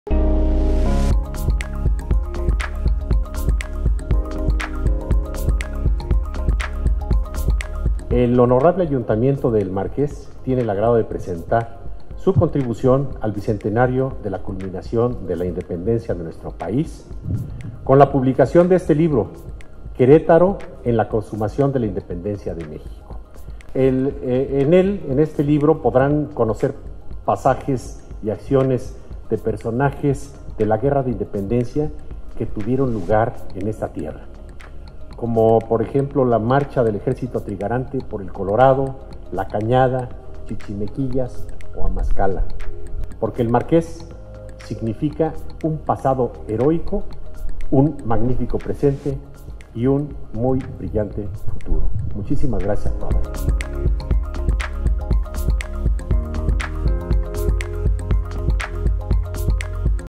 Presentación del libro "Querétaro en la consumación de la Independencia de México"
Presentó el Municipio de El Marqués "Querétaro en la consumación de la Independencia de México", edición conmemorativa del Bicentenario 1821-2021